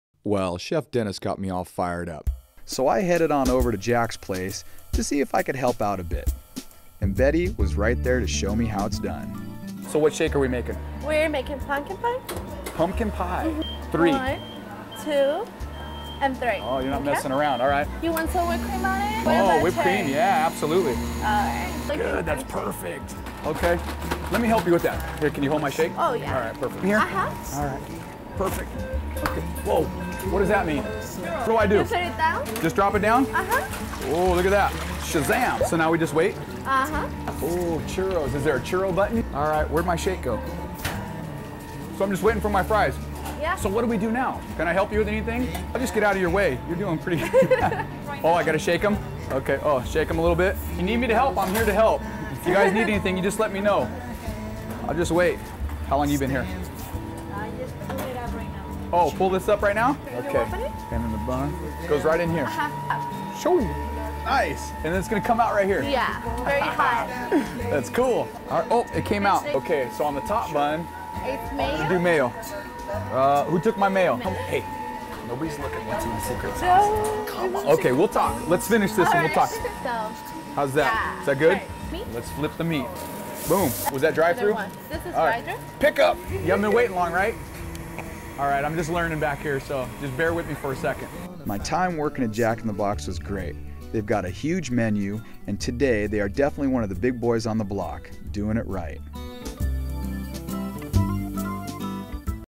JIB.mov
Wonderland host Noah Tafolla helps out at Jack in the Box